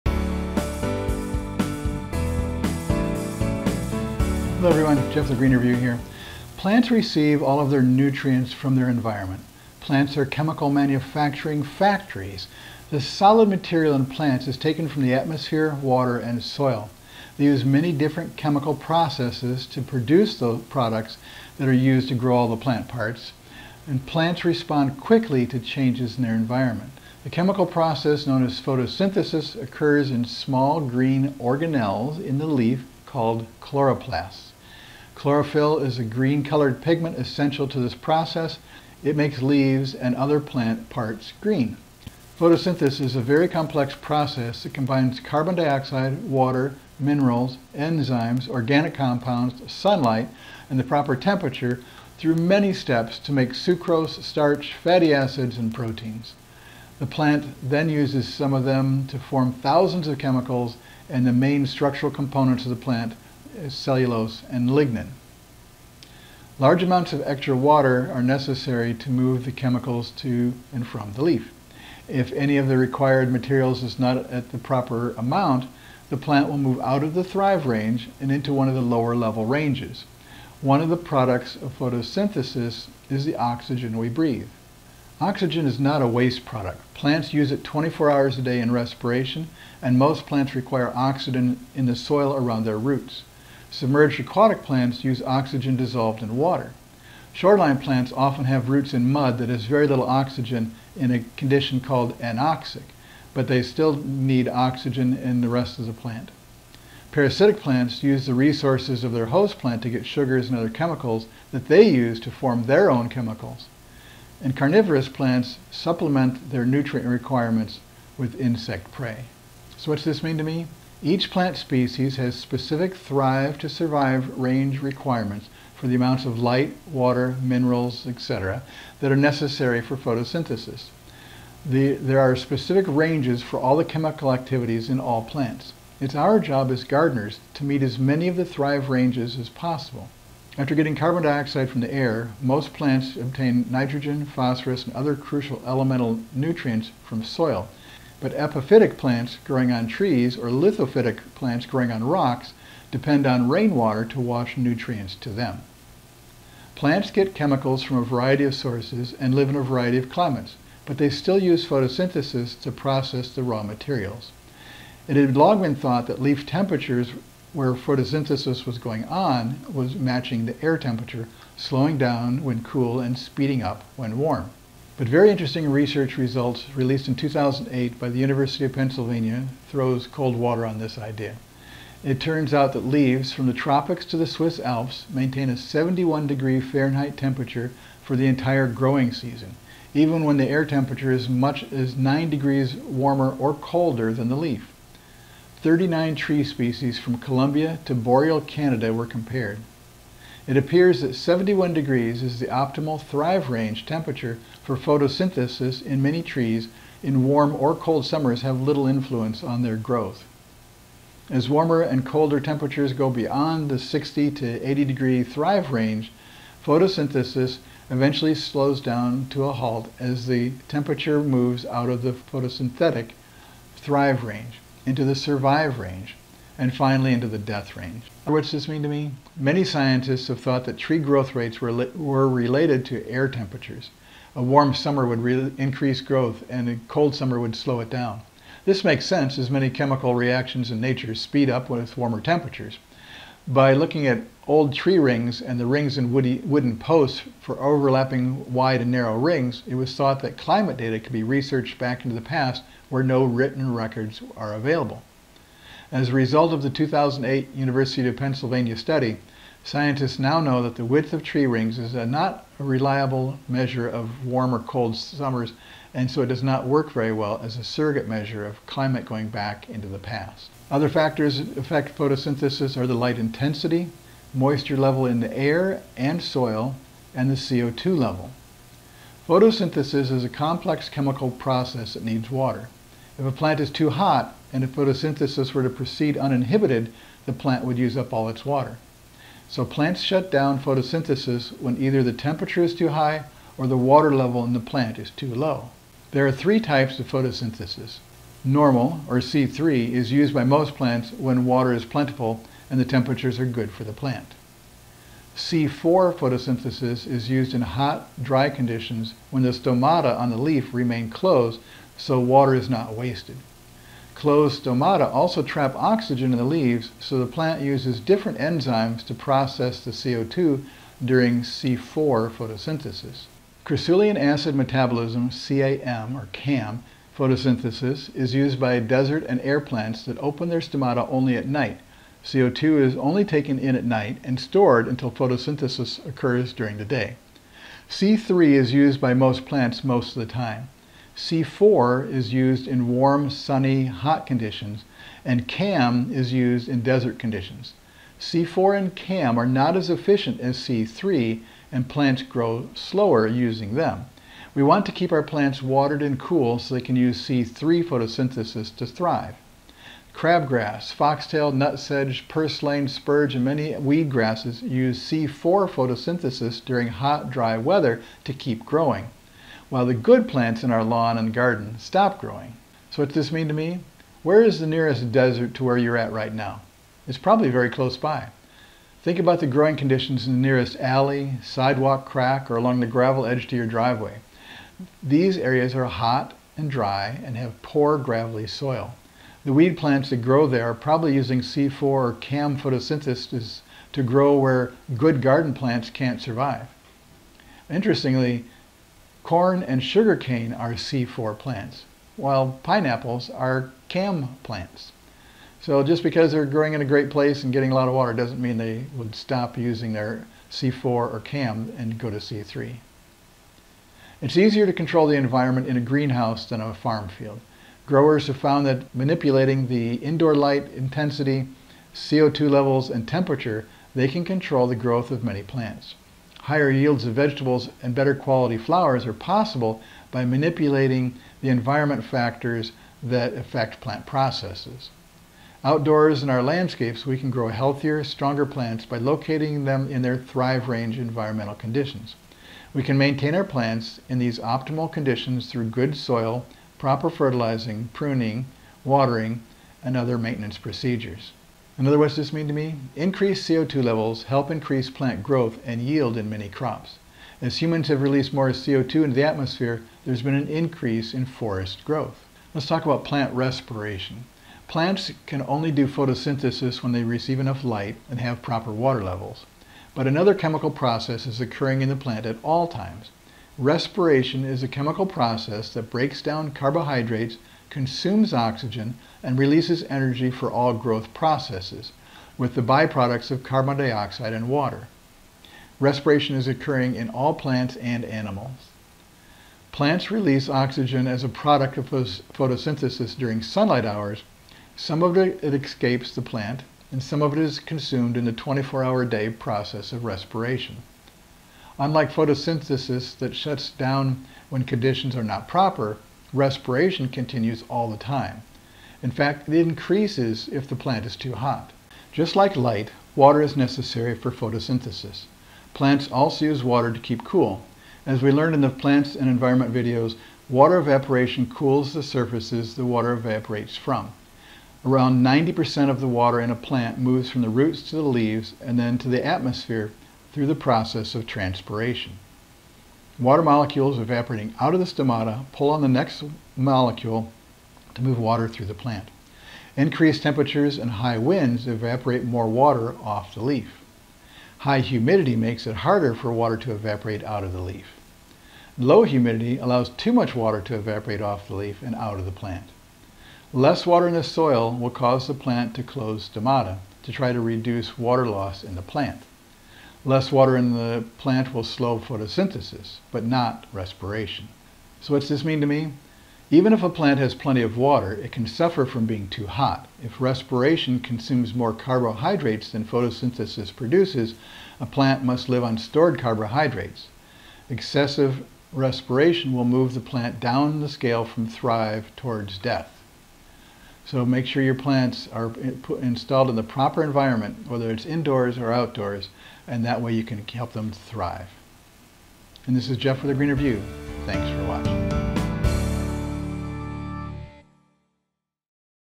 03 01 Photosynthesis and Respiration
As we saw in the previous chapter, plants receive all of their nutrients from their environment. Plants are chemical manufacturing factories. Photosynthesis is a very complex process that combines carbon dioxide, water, minerals, enzymes, organic compounds, sunlight and the proper temperature through many steps to make sucrose, starch, fatty acids and proteins. The plant then uses them to form thousands of chemicals and the main structural components of the plant, cellulose and lignin.
Respiration is a chemical process that breaks down carbohydrates, consumes oxygen and releases energy for all growth processes, with the by-products of carbon dioxide and water. Plants CONSUME oxygen 24 hours a day.